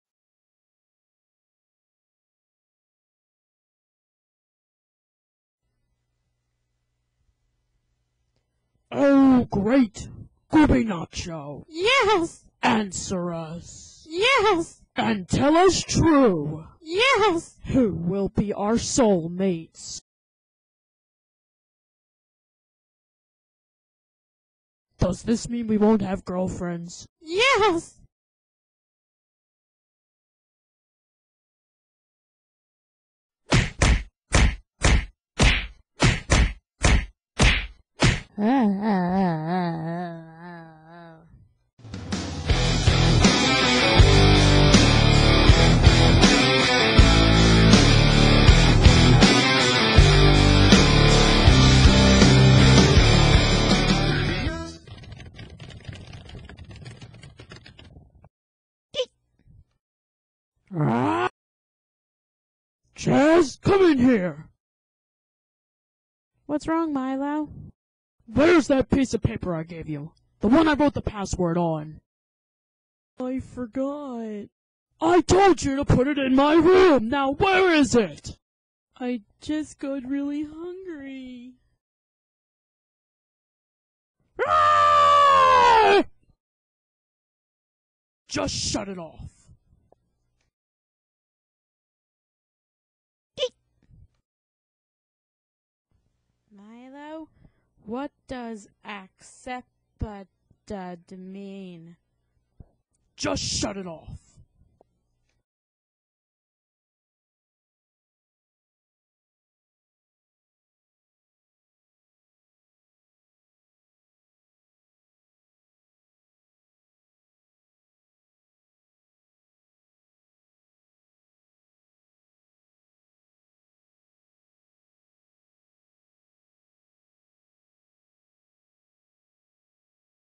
Gooby Nacho Ep 1
A short cartoon about 2 idiots who worship a nacho.